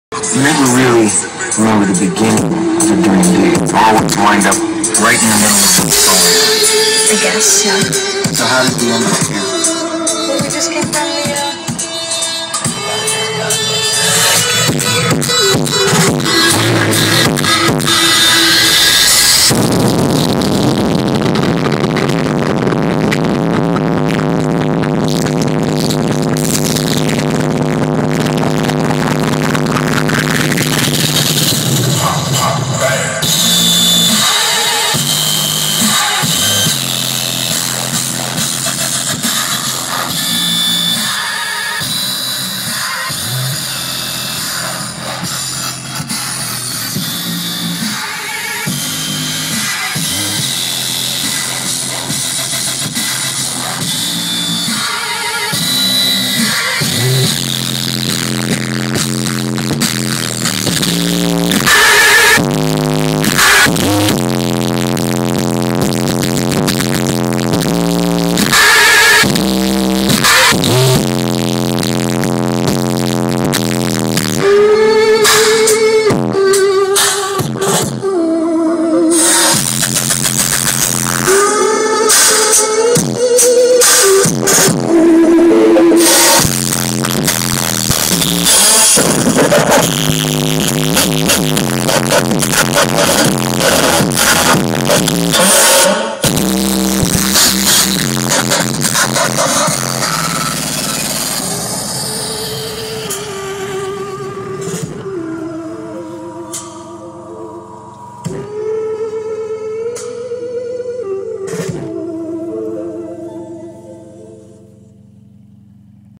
Dubstep highs test
Just testing the highs and showing some woofer porn